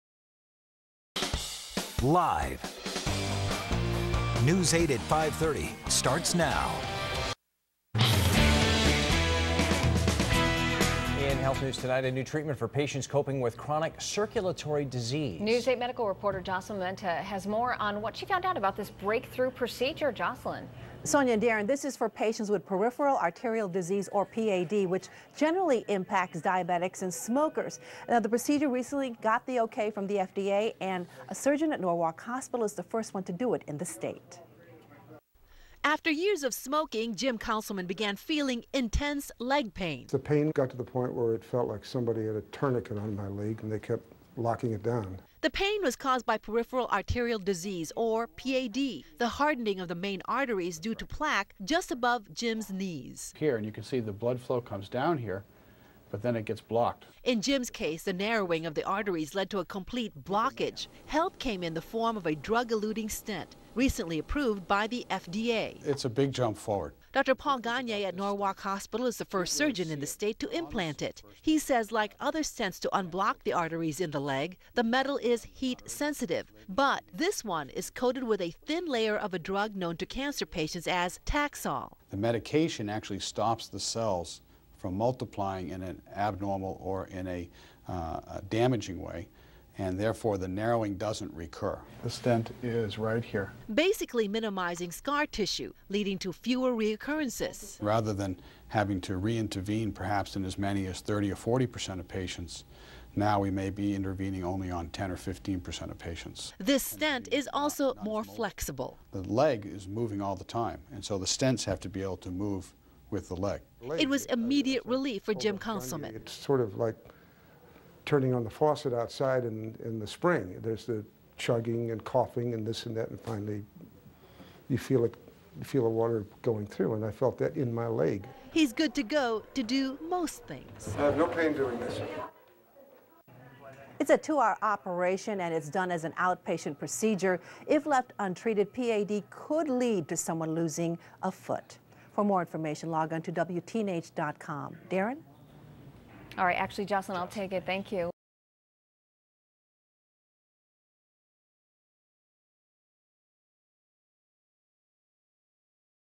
Life saving procedure for PAD
Peripheral Areterial Disease (PAD), a chronic circulatory disease in which plaque (i.e., atherosclerosis) forms in the arteries affects 8 to 12 million Americans each year. Over time plaque build-up can harden and narrow the arteries and block blood flow to the legs and other parts of the body. People with PAD can experience leg pain as well as serious complications including skin ulcers and gangrene. Until now, treatment options for PAD included lifestyle changes, medication, exercise, angioplasty, bare metal stenting and bypass surgery.